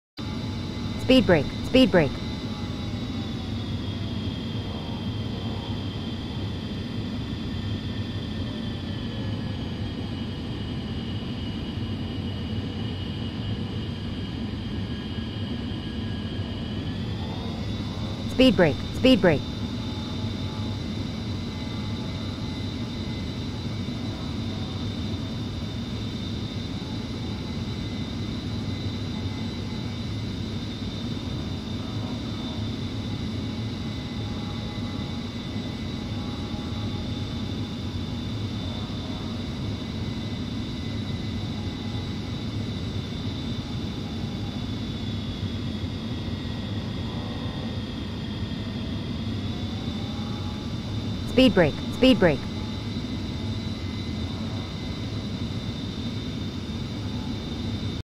Stall Warning Stick Shaker with FFB Stick
Stall Warning Stick Shaker.
Stick shaker operation is a function of AOA vane, AOA indicator and interlocks with landing gear position and flaps switch.